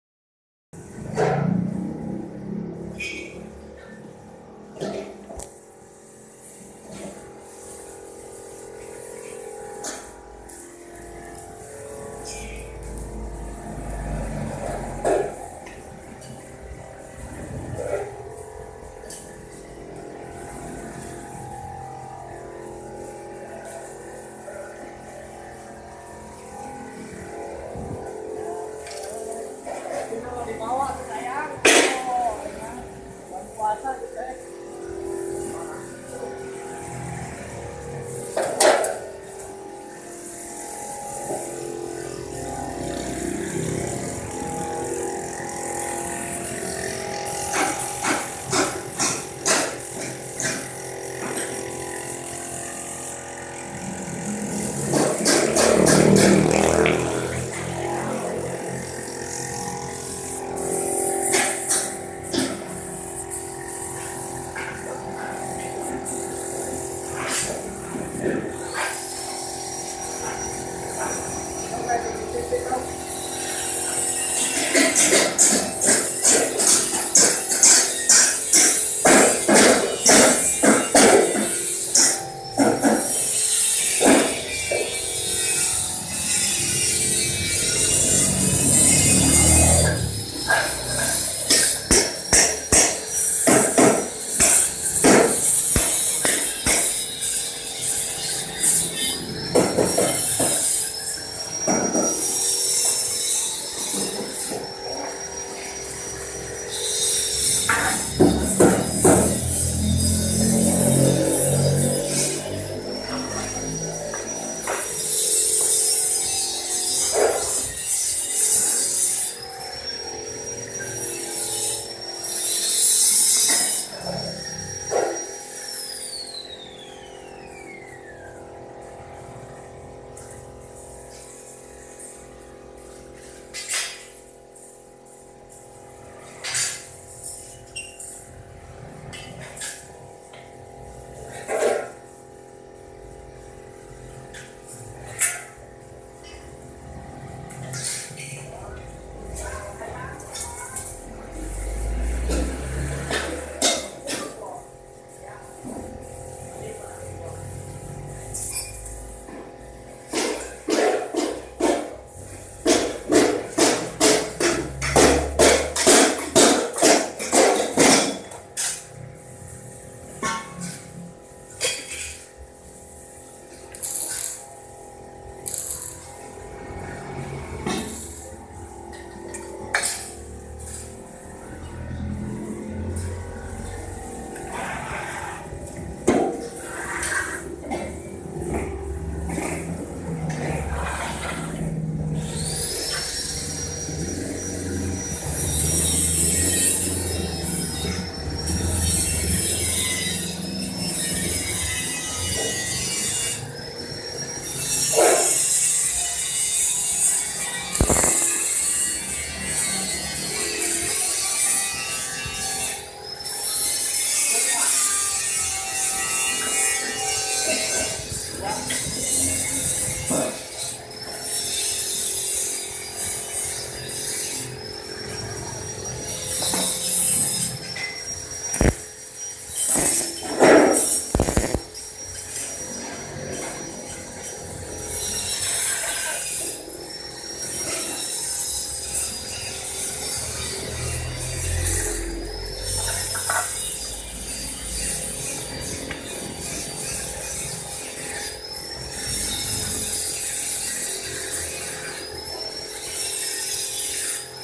kesibukan di workshop mitra
tukan muda berpengalaman,pasang baja pasang panel beton ringan.beton ringan
qpannel dak dinding